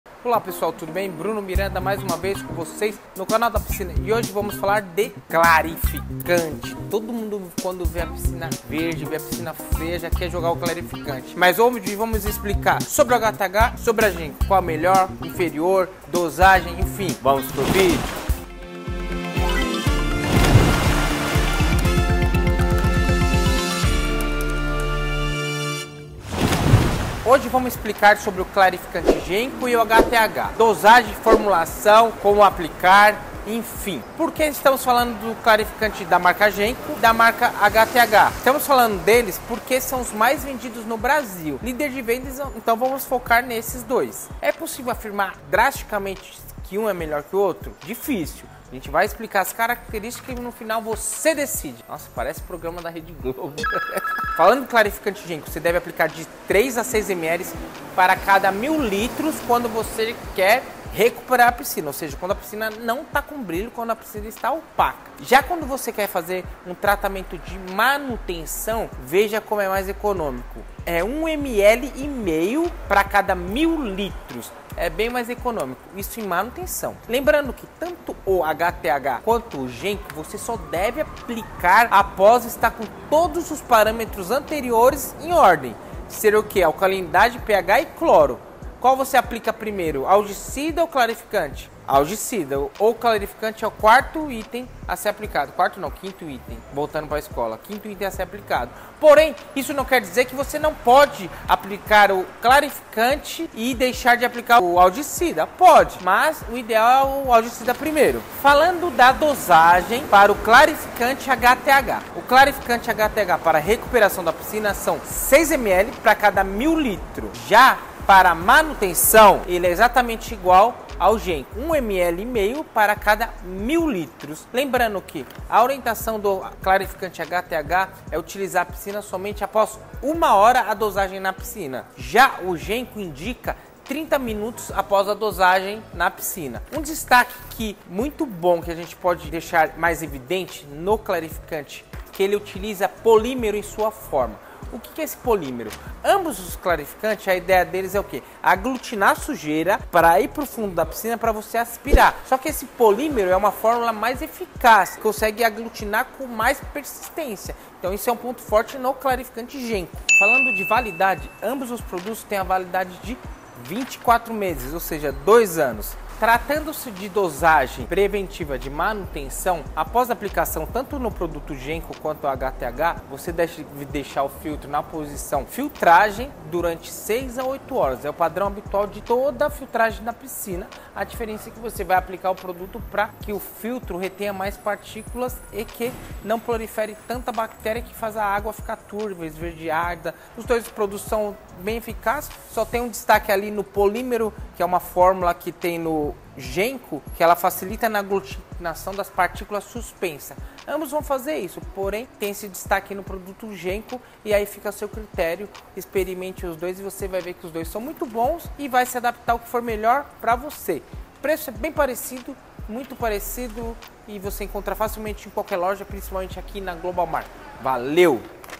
SAIBA A DIFERENÇA ENTRE OS CLARIFICANTES DE PISCINA: HTH OU GENCO?
Você possui uma piscina e quer mantê-la sempre cristalina e convidativa? Então, entender a importância dos esclarecimentos é essencial!
Neste vídeo, vamos explorar a diferença entre os clarificantes da HTH e da Genco, duas marcas conhecidas no mercado, para que você possa fazer a escolha certa e desfrutar de uma piscina deslumbrante durante toda a temporada!

Conheça a nossa loja:

Avenida Cruzeiro do Sul, 2318 - São Paulo - SP